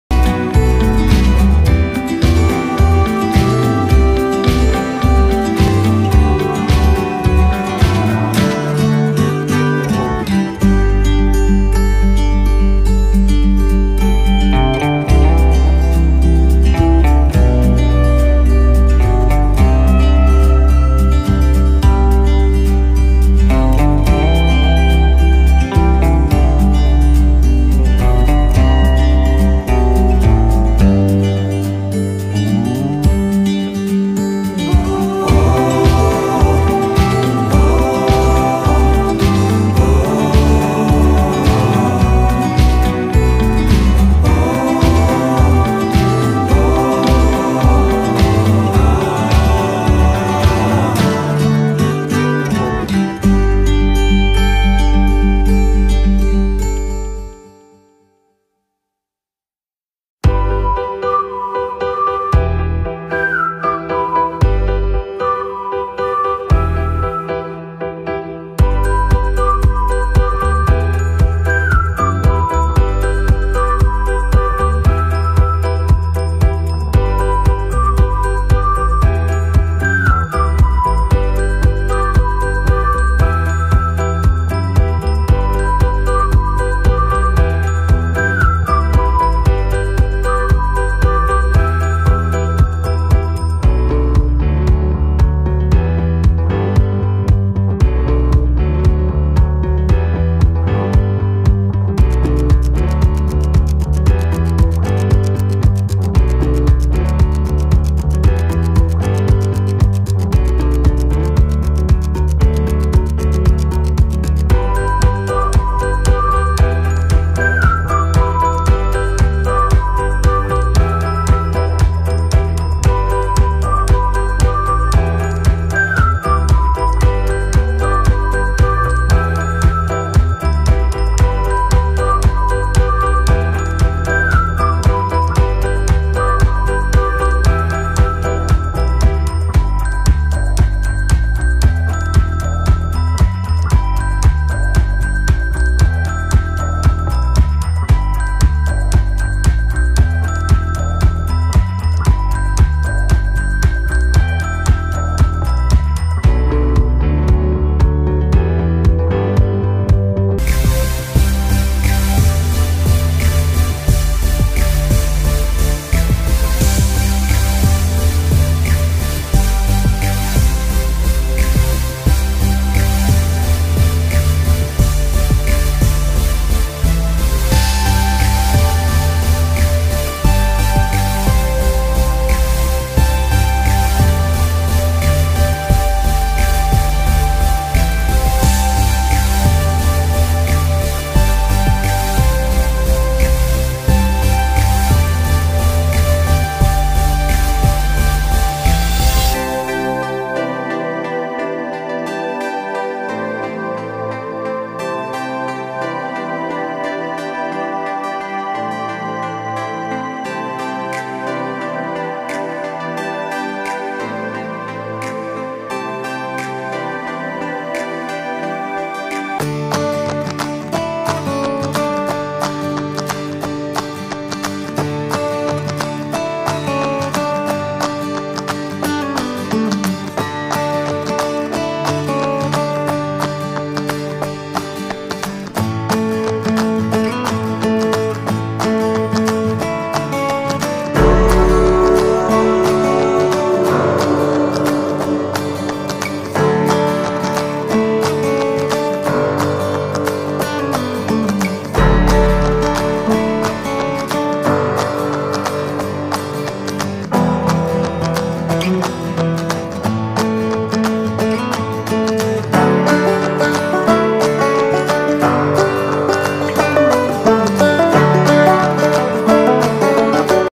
Pick holder
My husband is playing guitar and he always was complaining that he was loosing picks so I created for him a pick holder :D - all picks in one place